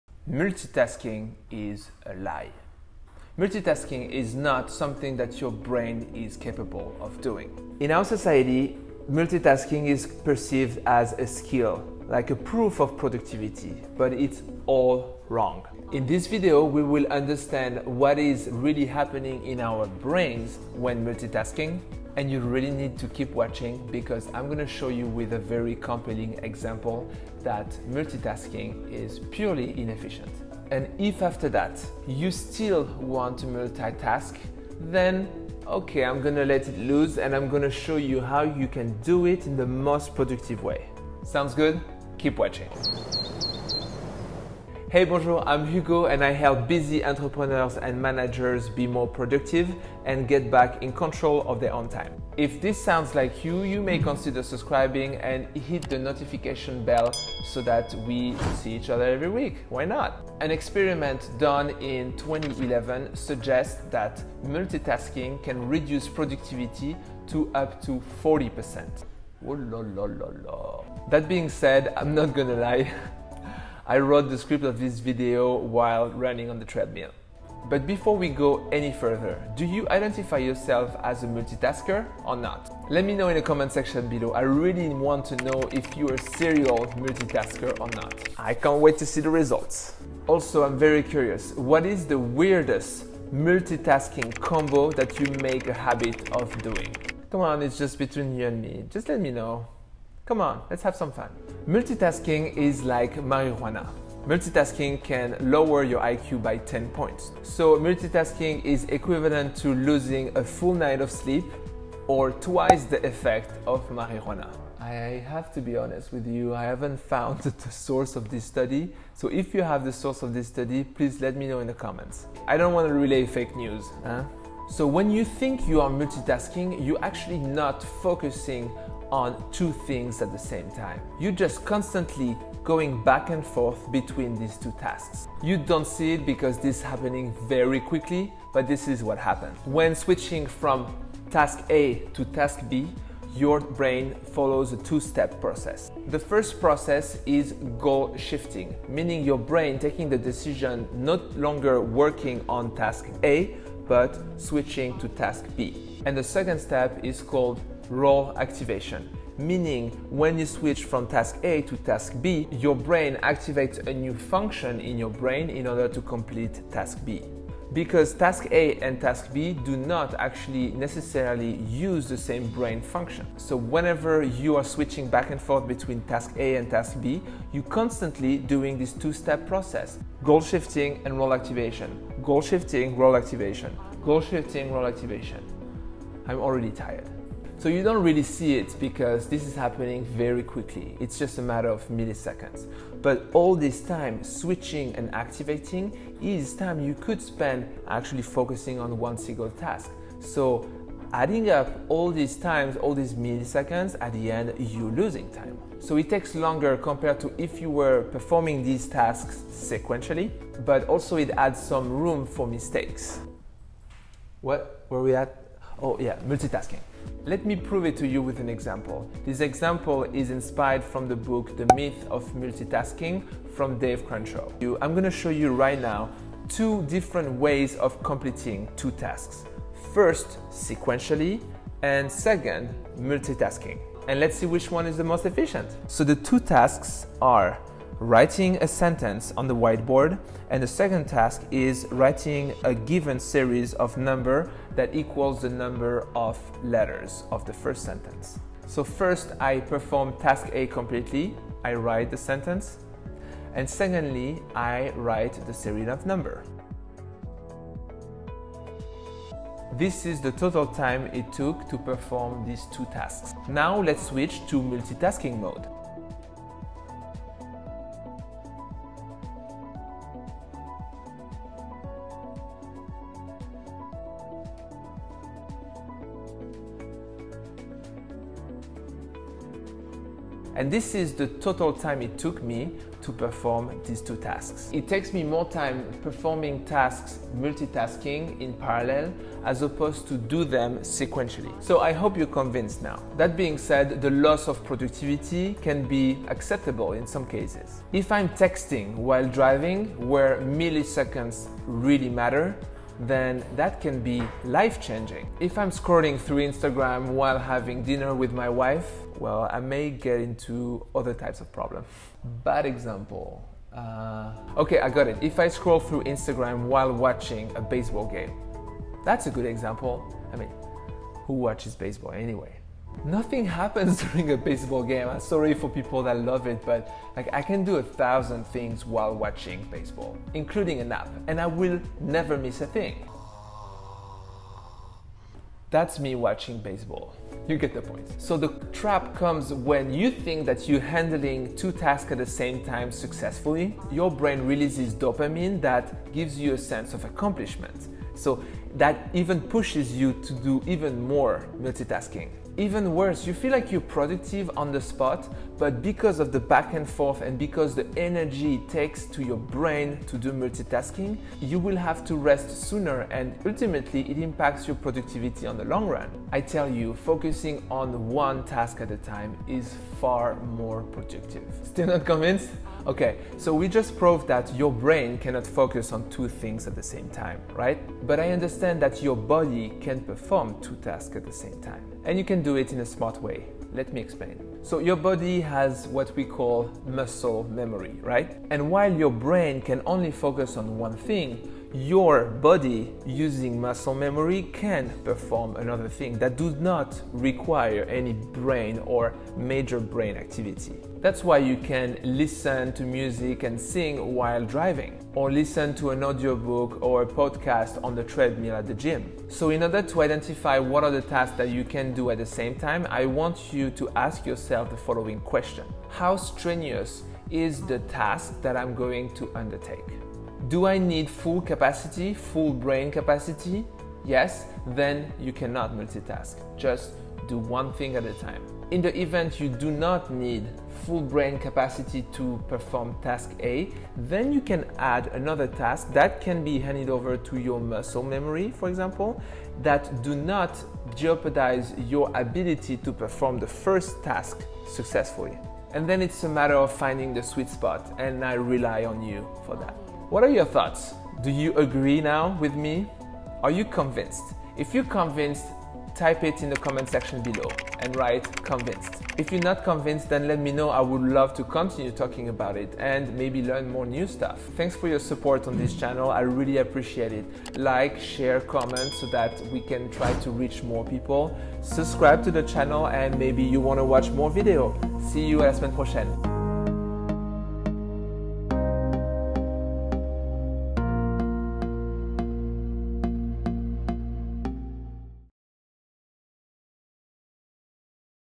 IS MULTITASKING A GOOD THING? HERE IS THE PROOF
In this video you will learn what your brain does when multitasking, and I am going to prove you with a compelling example how inefficient multitasking is. Ultimately, if you really want to multitask, I will show you how to do it in the most efficient way possible

DETAILS
Welcome to Time Flies Time Management Tips and Strategies, the channel that will help you get back in control of your own schedule and save time for things that really matter to you.